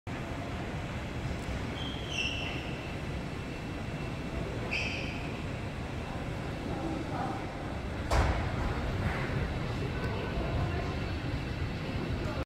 1 1
코엑스 스타필드 미디어윌이 멈췄다. [영상=정수남 기자]